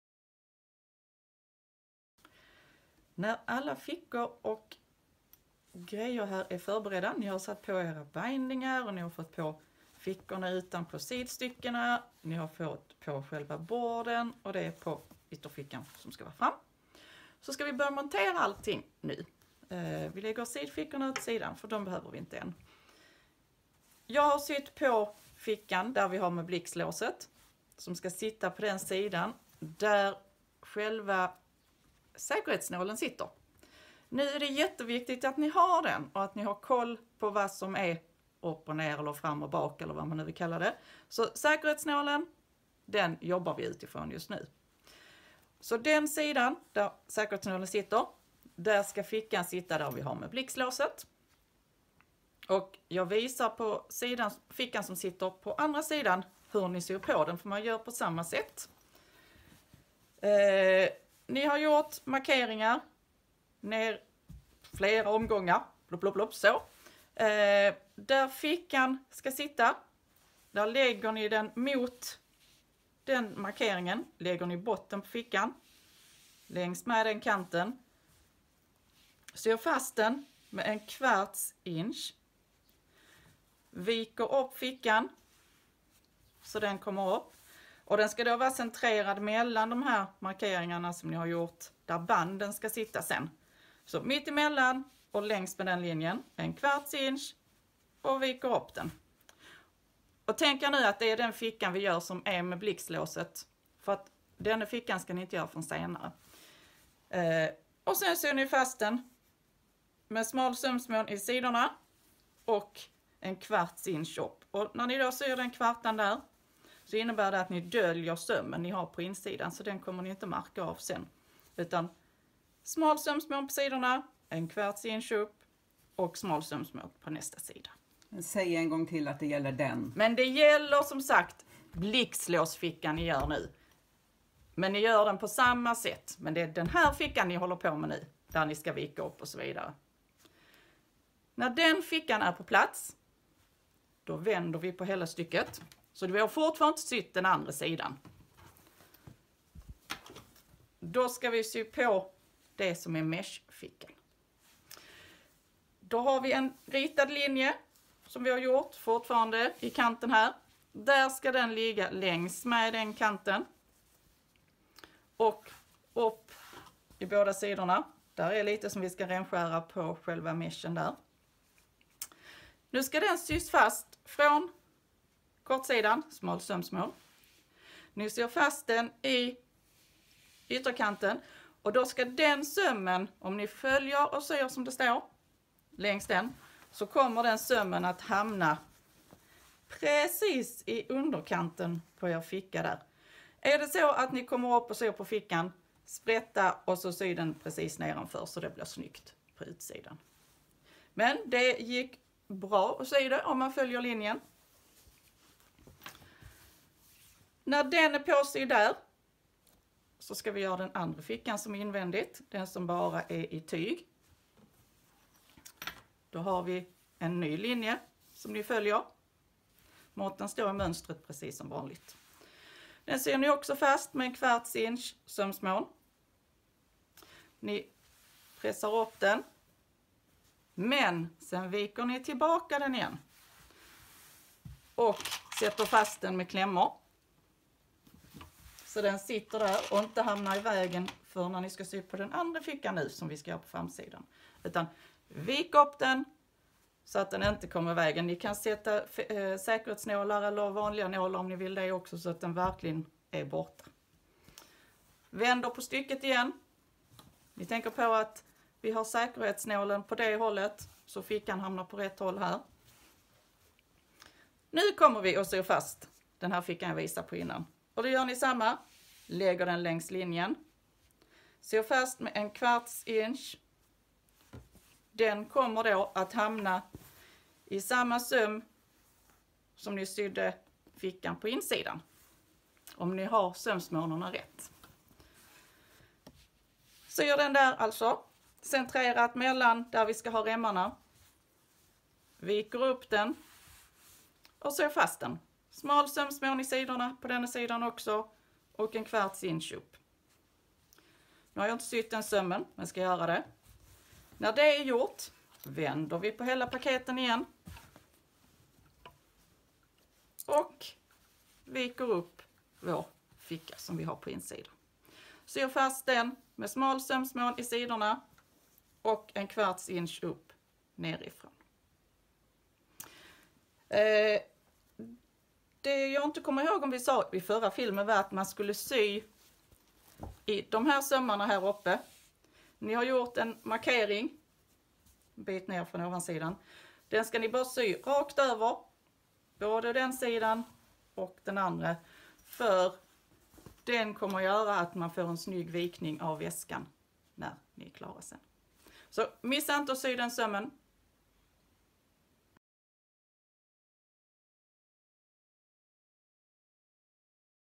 Get Out of Town Duffle 2.0 - 4
Del 4 av 5  i hur man syr Get Out of Town Duffle 2.0 från ByAnnie.

Detta är inga fullständiga instruktioner utan snarare tips och trix hur man kan göra.